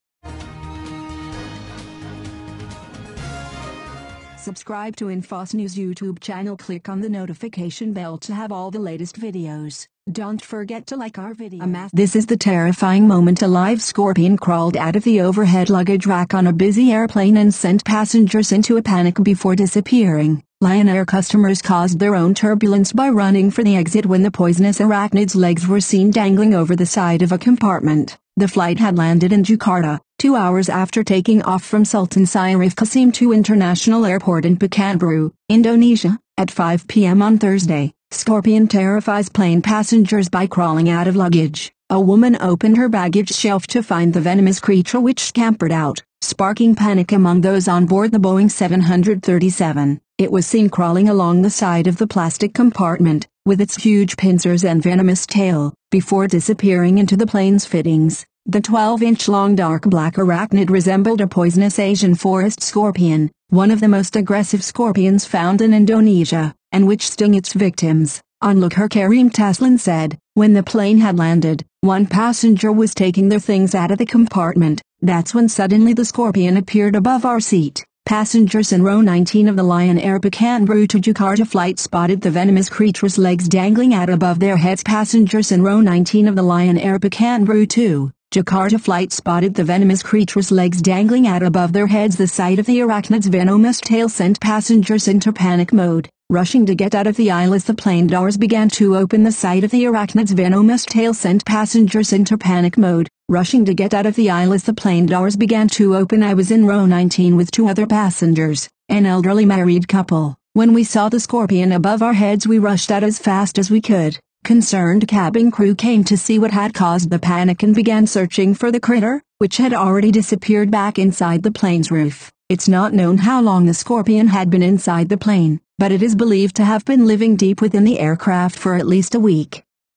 Scorpion terrifies Lion Air flight passengers after crawling out of the overhead luggage compartment
This is the terrifying moment a live scorpion crawled out of the overhead luggage rack on a busy aeroplane and sent passengers into a panic before disappearing.

Lion Air customers caused their own turbulence by running for the exit when the poisonous arachnid's legs were seen dangling over the side of a compartment.

The flight had landed in Jakarta, , two hours after taking off from Sultan Syarif Kasim II International Airport in Pekanbaru, Indonesia, at 5pm on Thursday.

Scorpion terrifies plane passengers by crawling out of luggage

A woman opened her baggage shelf to find the venomous creature which scampered out - sparking panic among those onboard the Boeing 737.

It was seen crawling along the side of the plastic compartment - with its huge pincers and venomous tail - before disappearing into the plane's fittings.

The 12-inch long dark black arachnid resembled a poisonous Asian forest scorpion - one of the most aggressive scorpions found in Indonesia - and which sting its victims.

Onlooker Karim Taslin said: 'When the plane had landed, one passenger was taking their things out of the compartment, that's when suddenly the scorpion appeared above our seat.

Passengers in row 19 of the Lion Air Pekanbaru-to-Jakarta flight spotted the venomous creature's legs dangling out above their heads
 The sight of the arachnid's venomous tail sent passengers into panic mode, rushing to get out of the aisle as the plane doors began to open
 'I was in row 19 with two other passengers, an elderly married couple. When we saw the scorpion above our heads we rushed out as fast as we could.'

Concerned cabin crew came to see what had caused the panic and began searching for the critter, which had already disappeared back inside the plane's roof.

It's not known how long the scorpion had been inside the plane, but it is believed to have been living deep within the aircraft for at least a week.